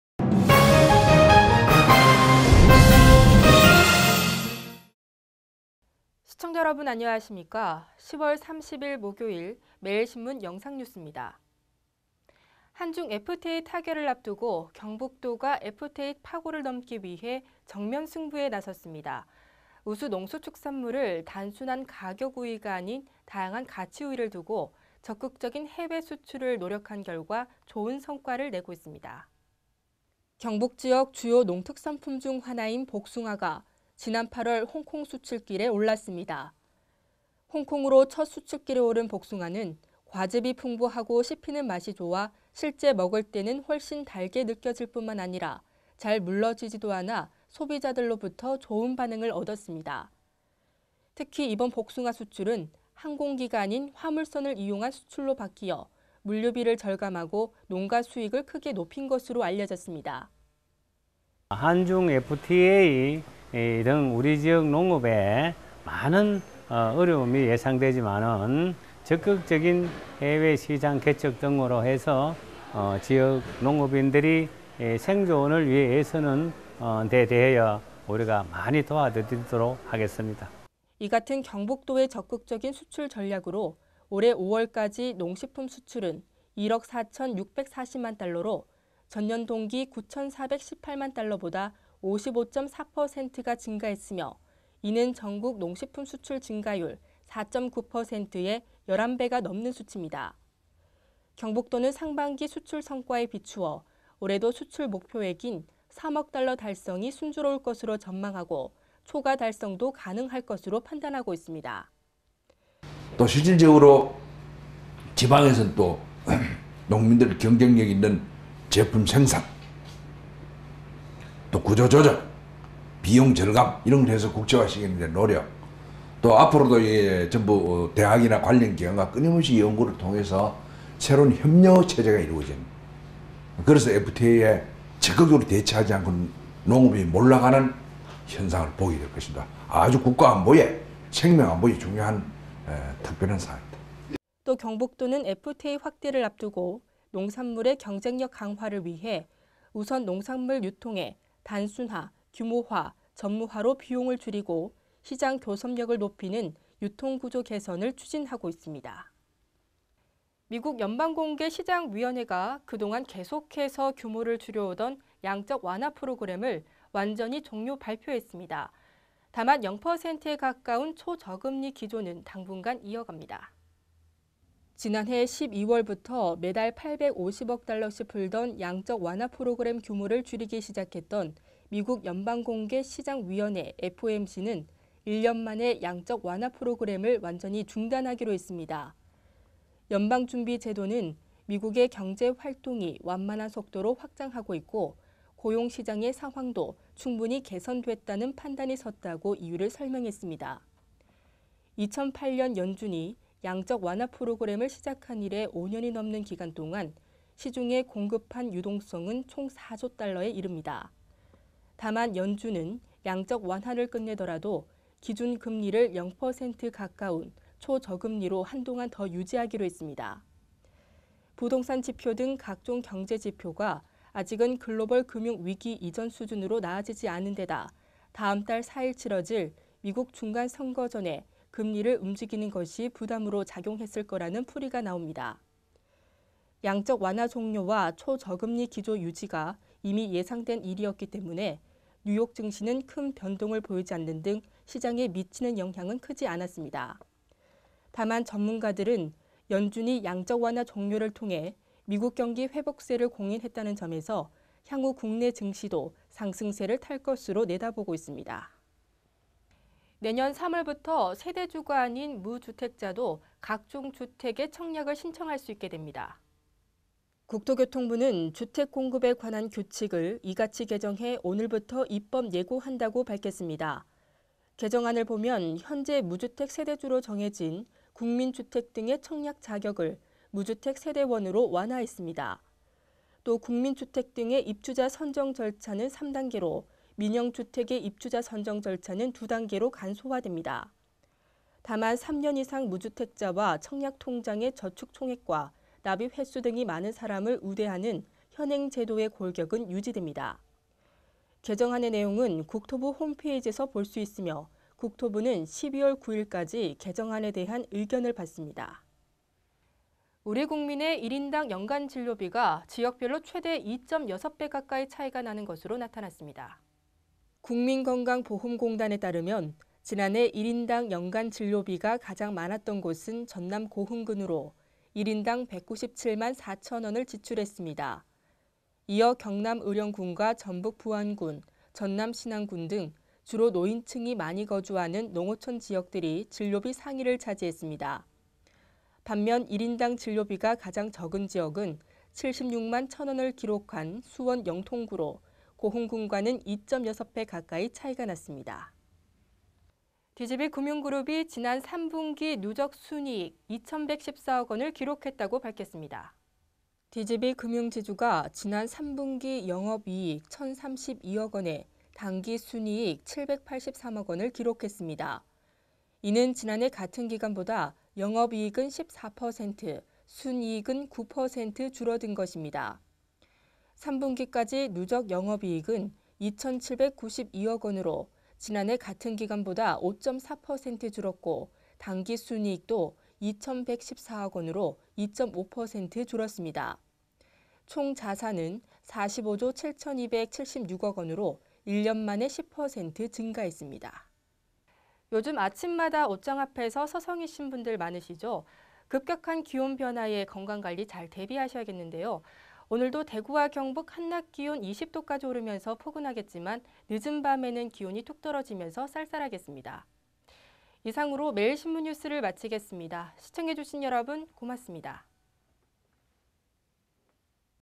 매일영상뉴스 1030
-경북도, 우수 농수축산물 수출로 FTA 정면승부
-美 “양적완화 종료” 발표…초저금리 유지
-내년 3월부터 무주택자 세대원도 청약 가능
-1인당 연간 진료비 지역별로 2.6배 격차
-DGB금융, 3분기 누적 순이익 2천114억원